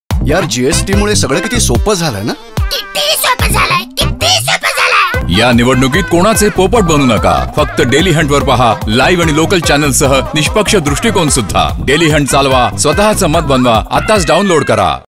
Marathi DH 15sec Office_या निवडणुकीत कोणाचा पोपट होऊ नका
निवडणुकांच्या हंगामाची चाहूल लागली आहे. आता तुमच्या आजुबाजूला अनेक पोपट फिरताना दिसून येतील. लक्षात ठेवा, ते कुणाच्या तरी मतांचा पुनरुच्चार करतील. जर तुम्ही अशा कुणाला ओळखत असाल, तर ही फिल्म त्याला दाखवा आणि त्याला सांगा